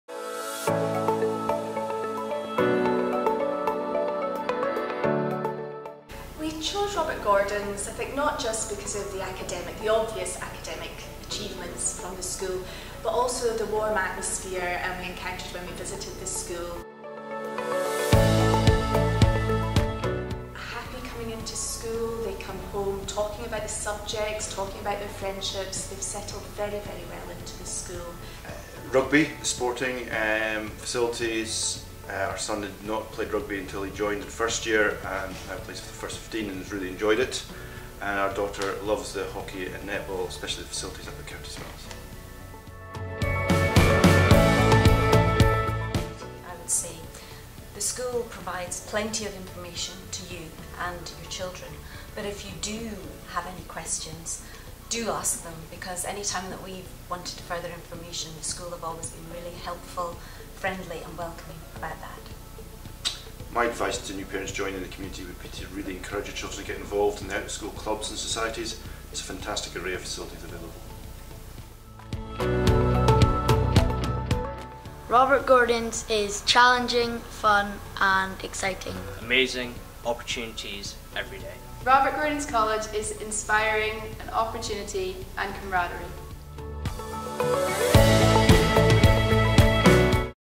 Why I chose Gordon's | Senior School | Robert Gordon's College
Parents from Robert Gordon's College Senior School share why they chose Gordon’s.